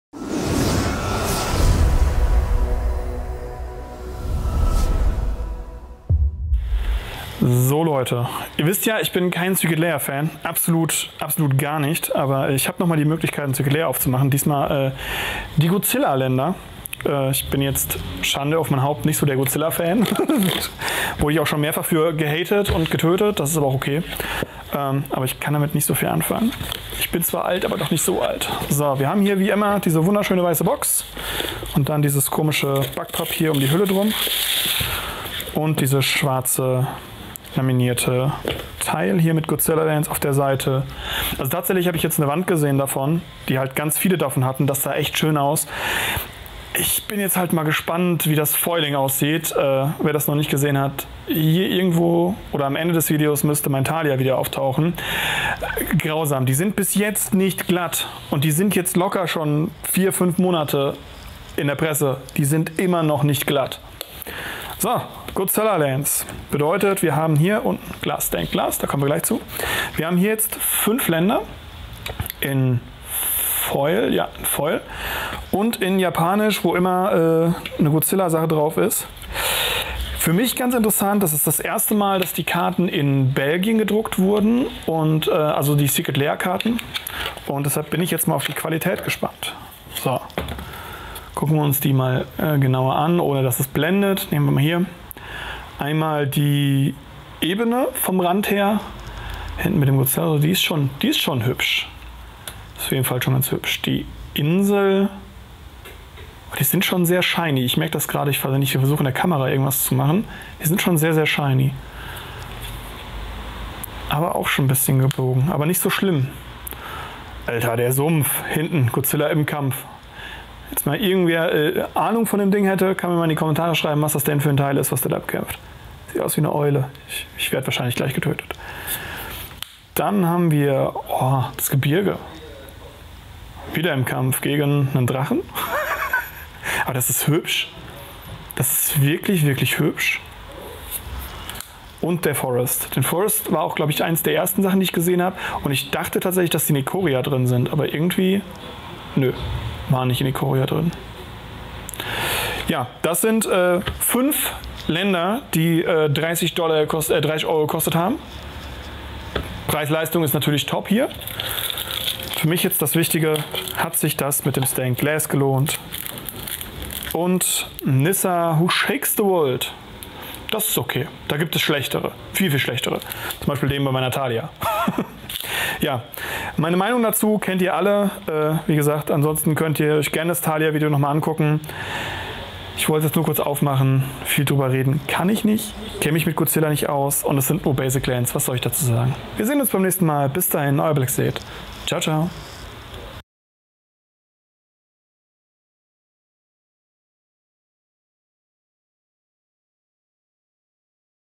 Blacky zeigt: Secret Lair Drops: Godzilla Lands
Auch wenn ich ein absoluter Secret Lair Gegner bin, um mich herum sind zu viele Leute die es mögen. Der Vorteil für euch? Ich kann es für euch auspacken und euch zeigen.